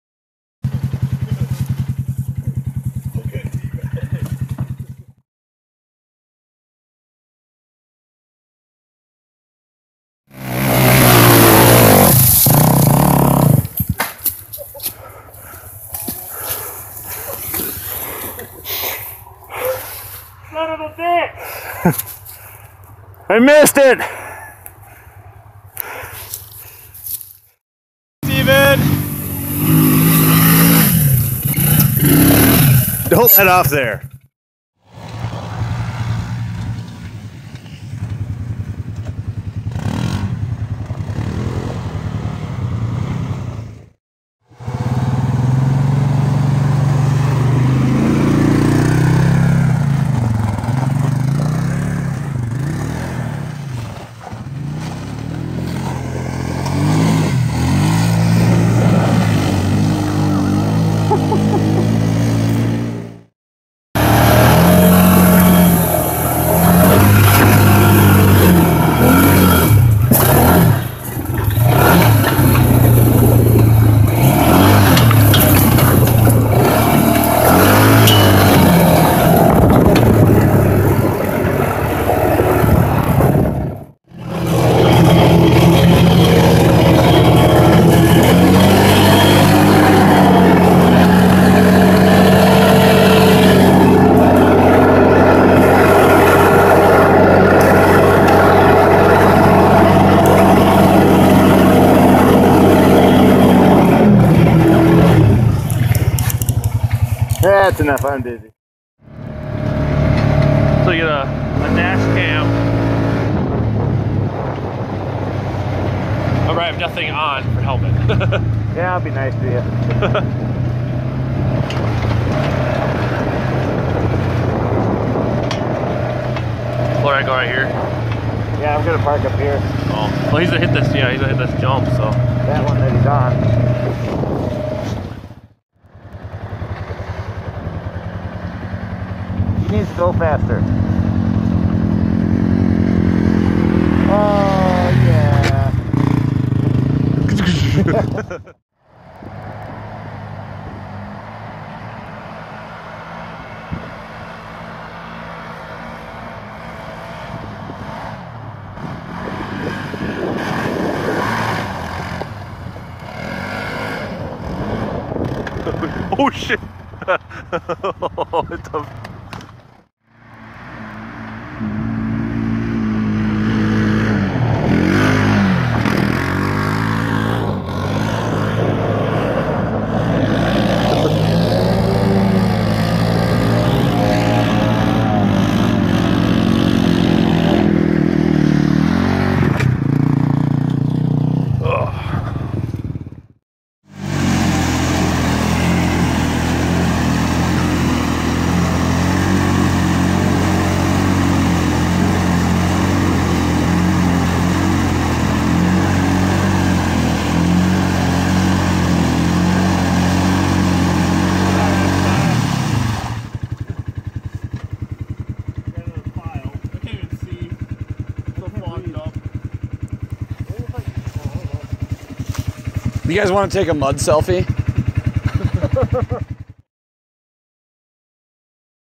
My Friends and I Took a Trip to Moose Lake, MN
2 Polaris RzRs, a Polaris Ace, and a Polaris Predator conquer the Soo Line Gravel Pits in late November.  It was cold and we got wet but it was totally worth it!